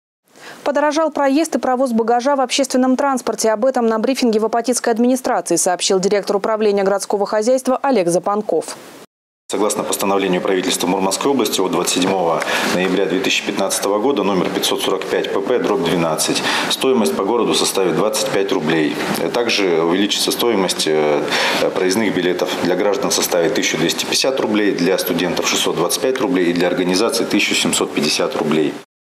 По городу за "четвертак"
Подорожал проезд и провоз багажа в общественном транспорте – об этом на брифинге в апатитской администрации сообщил директор Управления городского хозяйства, Олег Запанков.